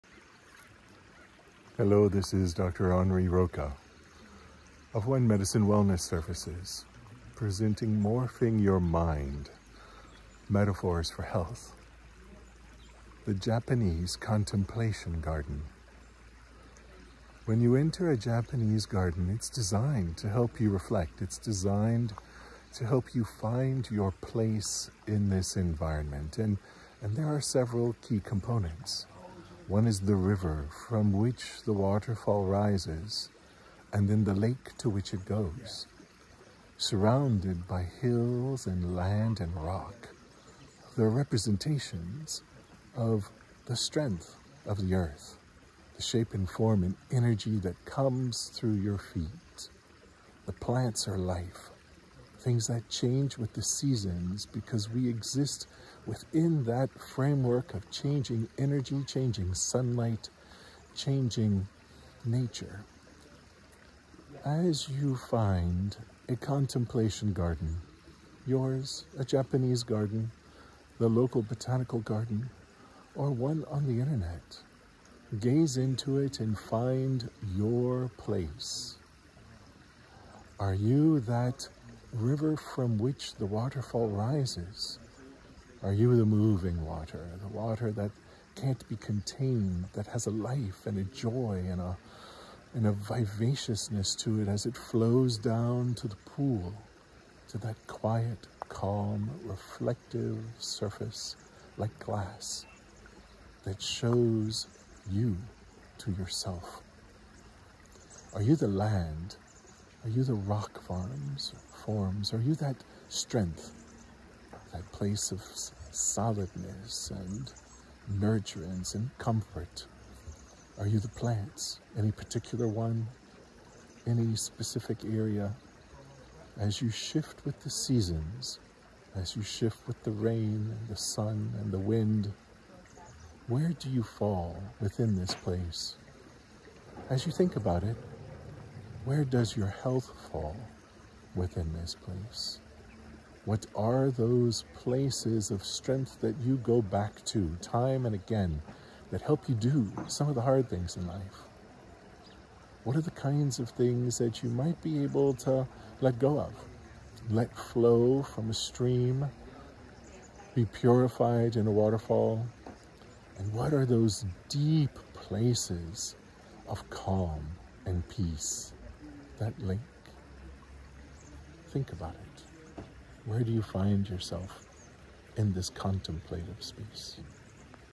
Metaphors For Health Japanese Contemplation Garden
Japanese Contemplation Gardens are designed to help you reflect & find your place in the environment.

They are created to represent the strength of the earth & an image of its beauty.

We encourage you to find YOUR garden.